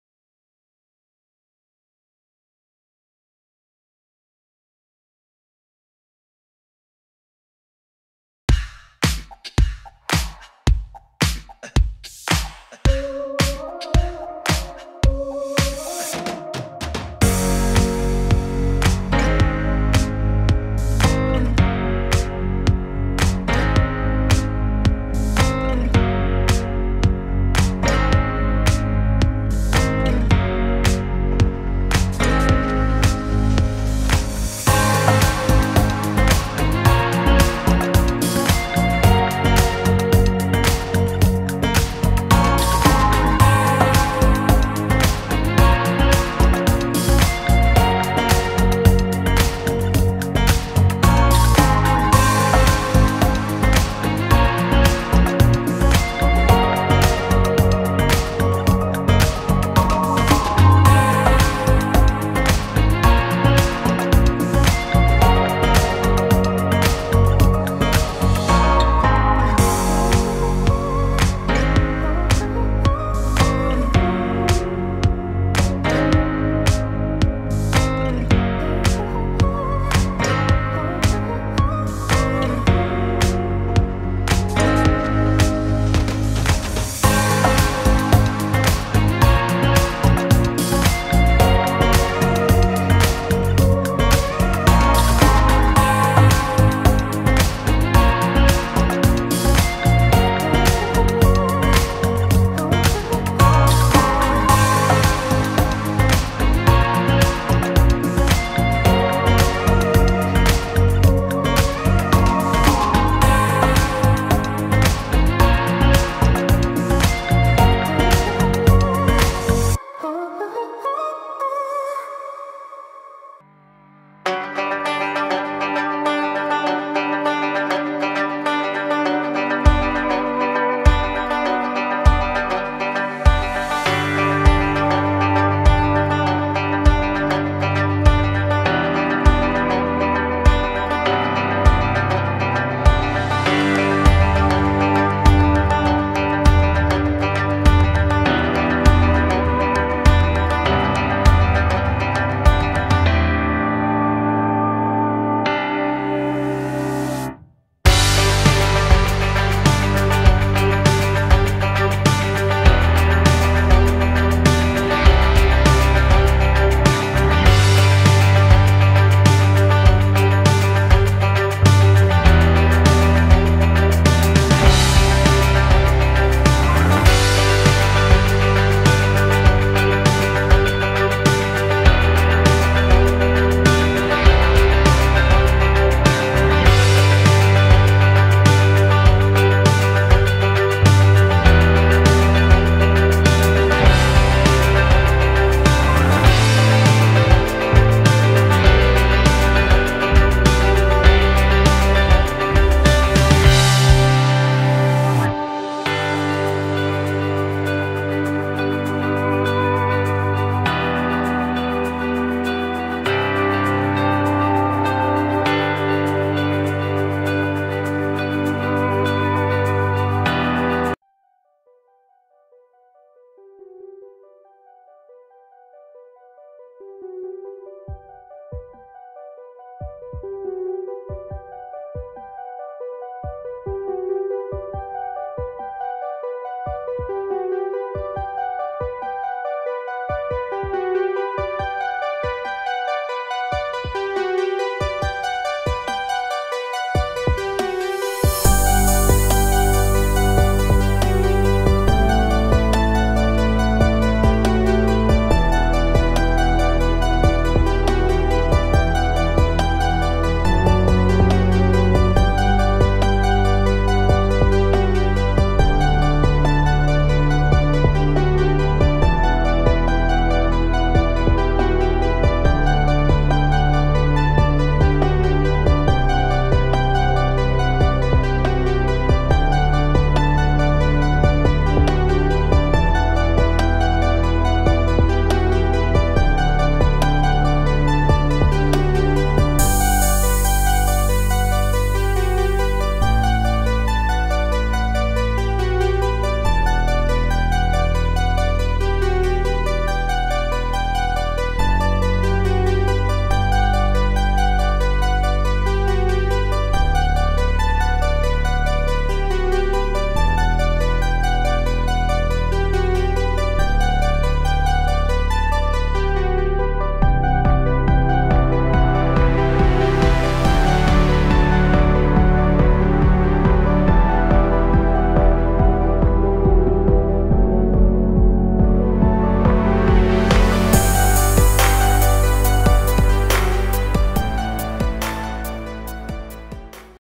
Honey cake homemade , Russian honey cake
Russian layered honey cake Recipe